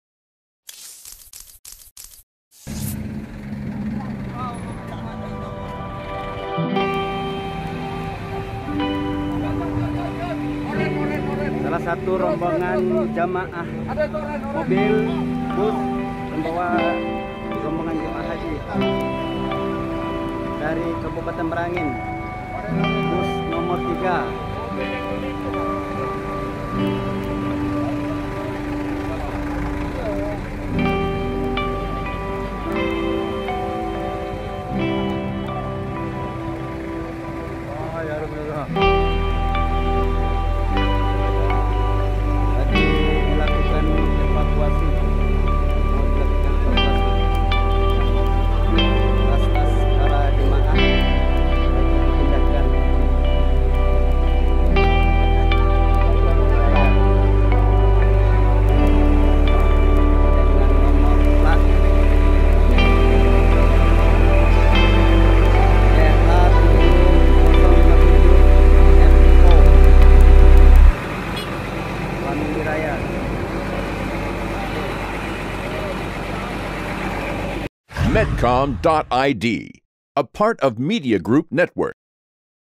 Bus Rombongan Calon Jemaah Haji Alami Kecelakaan di Jambi
Beginilah kondisi mobil bus yang terposok kedalam parit, mobil keluar dari jalur pembatas ruas jalan setelah mengalami insiden kecelakaan tunggal. Kejadian ini berlangsung, di Jalan Lintas Tembesi, Batanghari, Jambi pada Minggu (26/6).

Dir lantas polda jambi, Kombes Pol Dhafi menyebut sebelum kejadian, rombongan bus yang membawa rombongan calon Jemaah haji berjumlah tiga mobil dari Kabupaten Merangin tersebut bertolak ke Kota Jambi. Namun di pertengahan perjalanan, salah satu dari tiga kendaraan itu hilang kendali karena menghindari kendaraan lain. Saat ini, mobil bus tersebut telah berhasil di evakuasi, dan jalur lintas sudah kembali normal dari kemacetan. Sementara puluhan Jemaah haji sudah dialihkan ke bus lain. Tak ada korban jiwa dalam peristiwa ini.